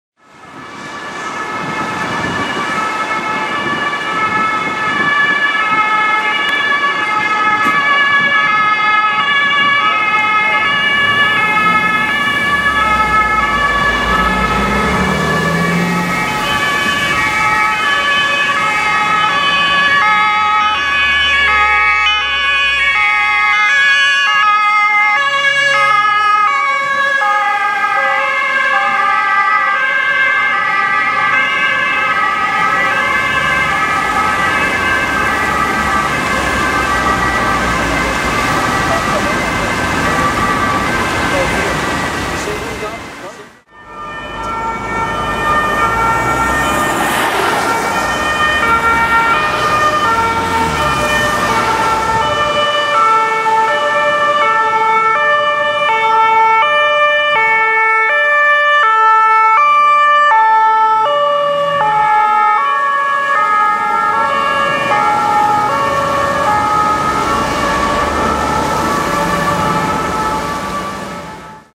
2x FuStW + GruKw Stadtpolizei Frankfurt
Zwei Funkstreifenwagen und ein Gruppenkraftwagen der Stadtpolizei Frankfurt auf Alarmfahrt. //

Two patrol cars and a van of the Frankfurt city police department responding. //

Frankfurt, Hessen, Deutschland (Germany). -04.2012-

by Mario(40029)